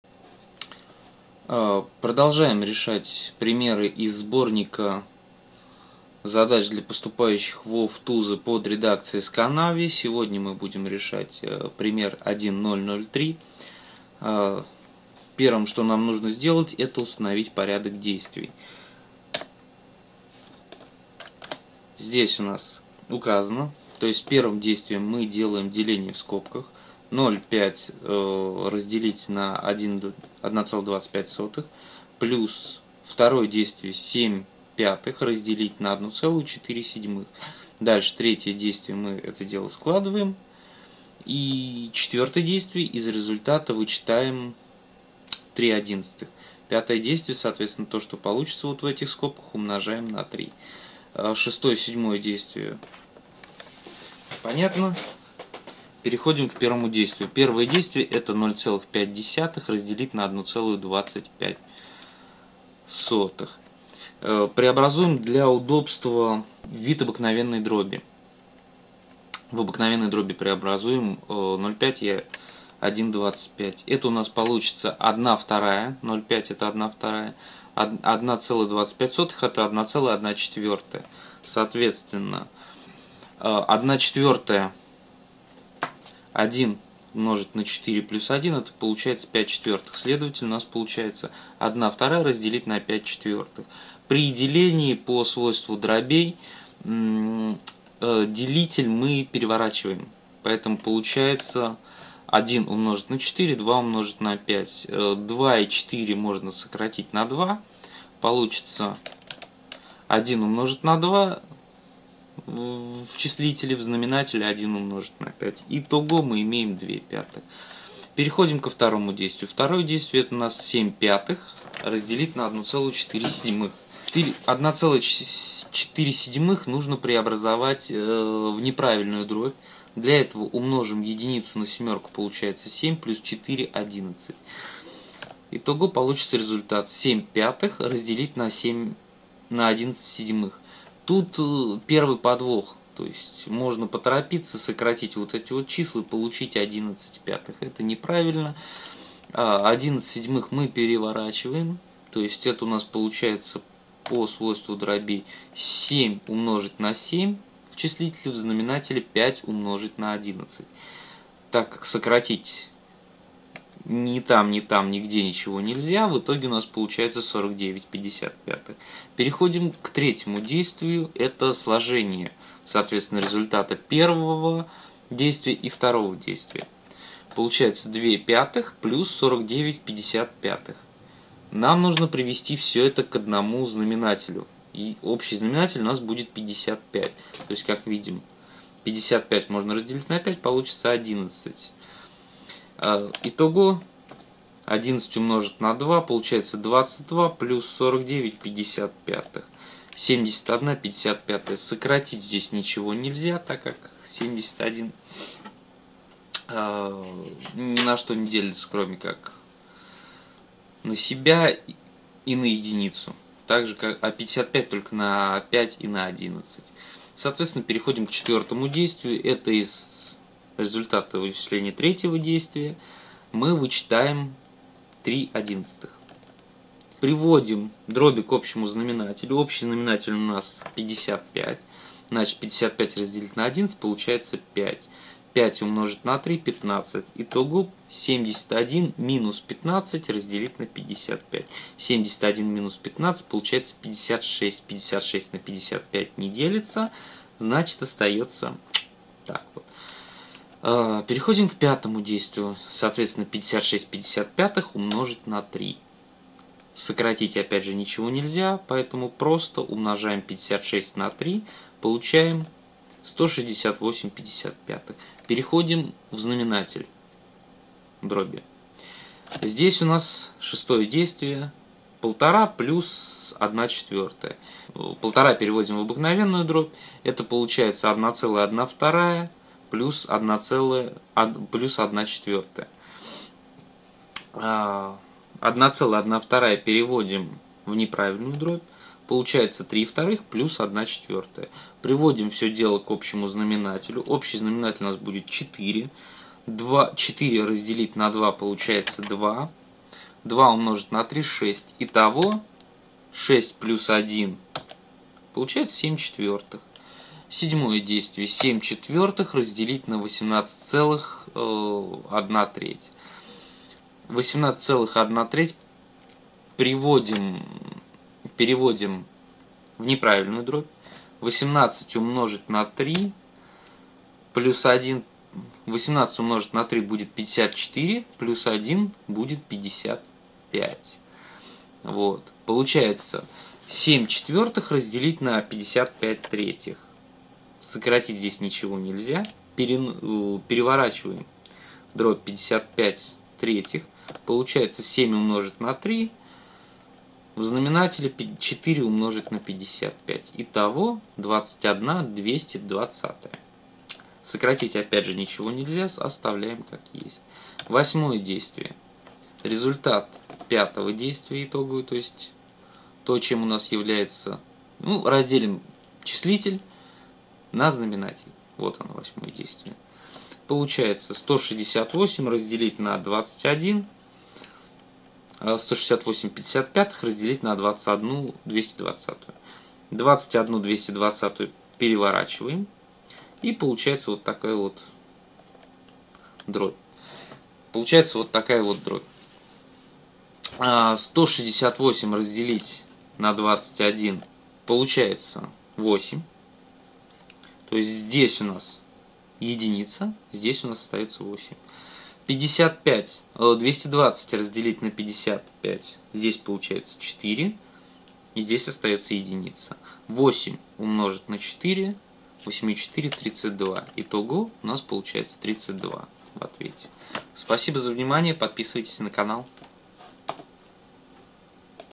Сканави 1.003
решение примера 1.003 из сборника задач по математике для поступающих во ВТУЗы под редакцией М.И.Сканави 6-е издание